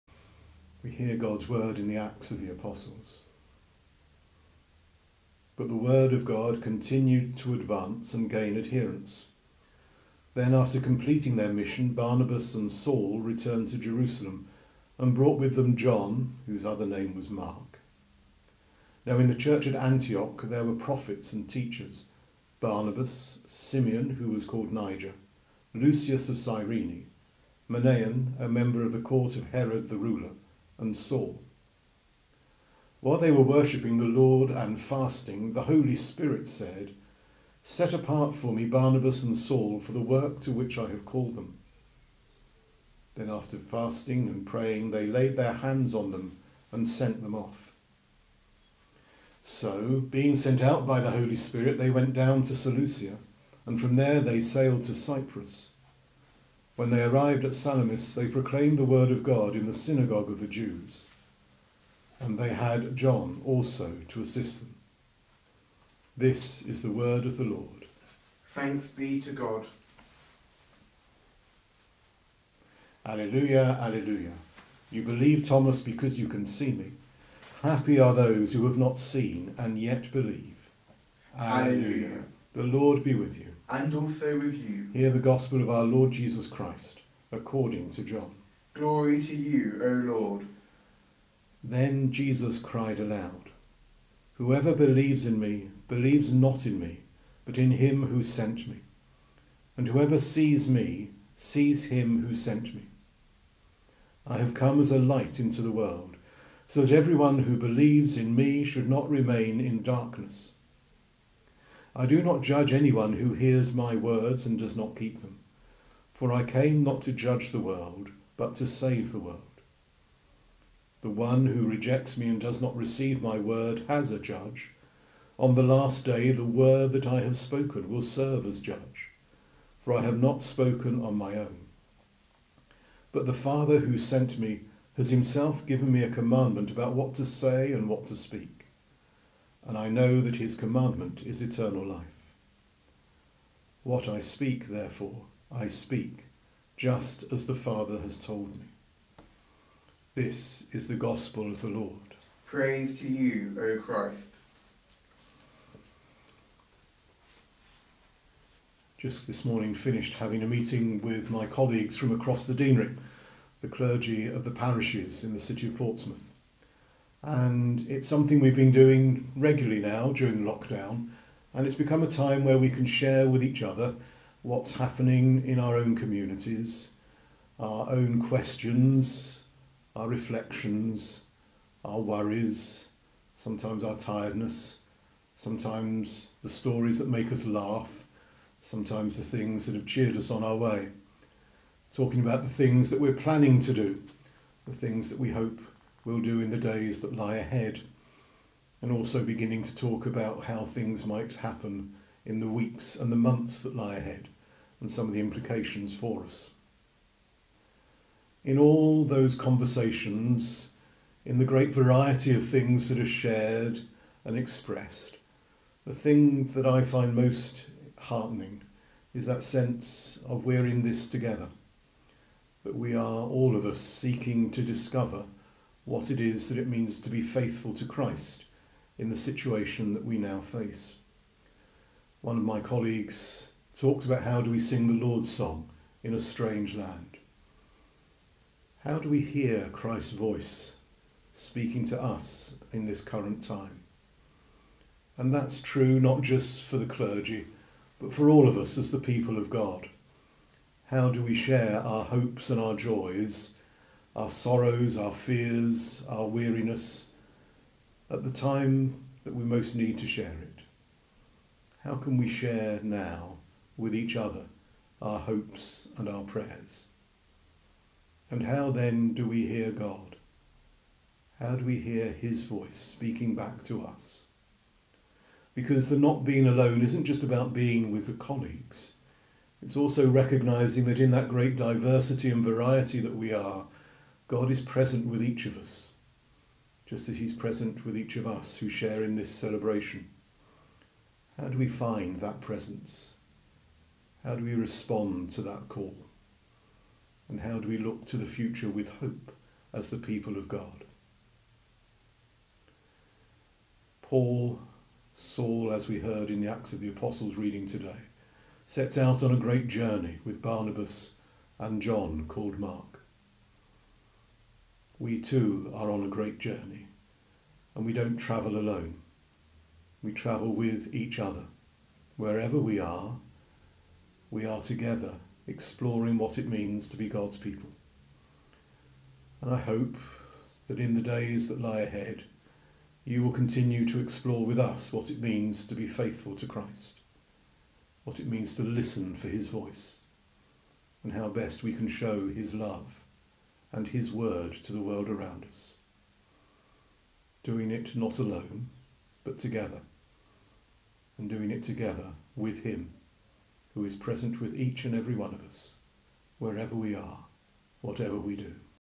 Daily Reflection - 6th May 2020 - St Mary's Church, Portsea
Started during the COVID-19 pandemic, this series aims to provide you with daily reflections from the parish priests based around the day’s gospels and world news.